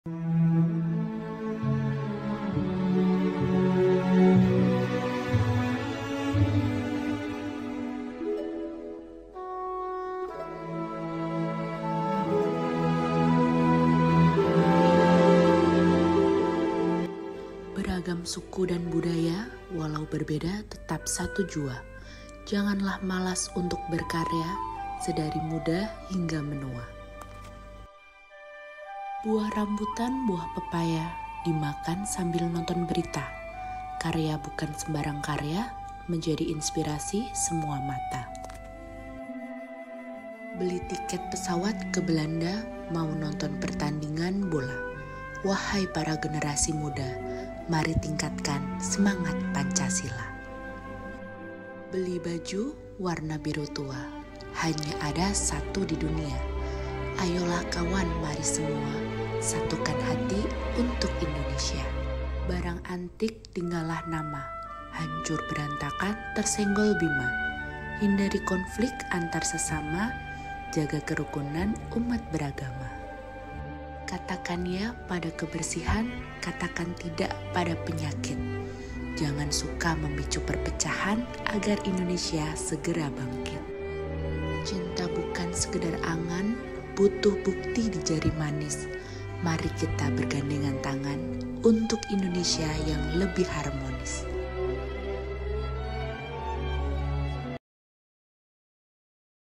LOMBA BERALAS PANTUN - SELARAS - CABANG LAMPUNG
Hai Jasfren dan Somi..
Dalam rangka menyambut HUT JR ke-63, PT Jasa Raharja menyelenggarakan Lomba Berbalas Pantun  bertema "Harmoni untuk Negeri" dengan total hadiah puluhan juta rupiah.

Yuk dukung karya-karya terbaik Insan Jasa Raharja dengan like dan comment ya..!!